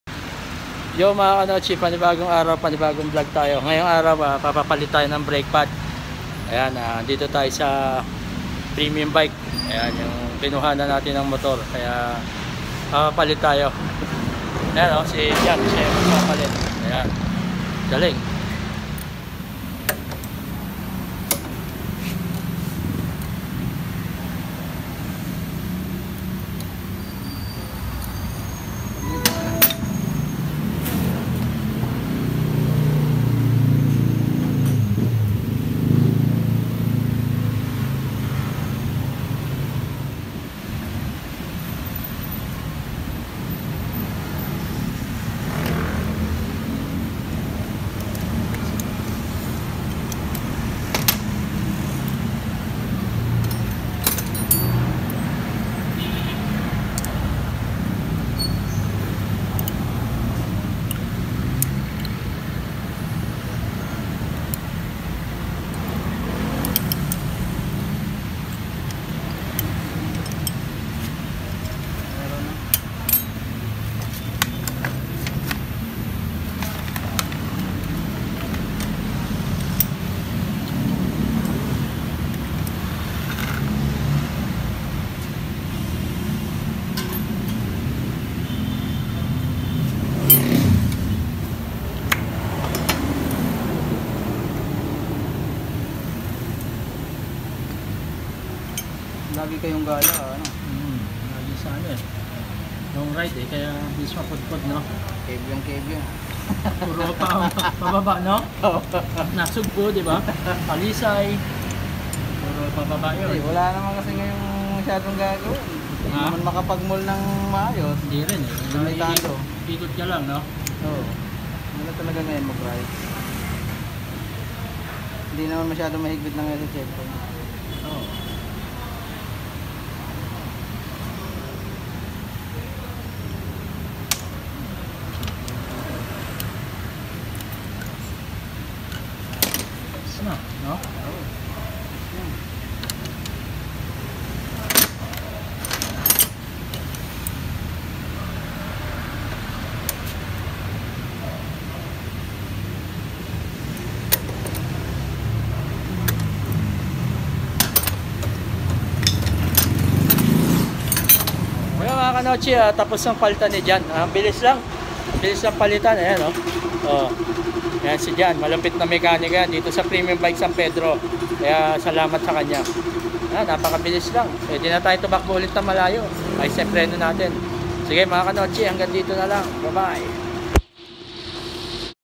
MIO i 125 S l HOW TO REPLACE BRAKE PAD OF MOTORCYCLE | Kanotchi TV | Vlog #42
MIO i 125 S l HOW TO REPLACE BRAKE PAD OF MOTORCYCLE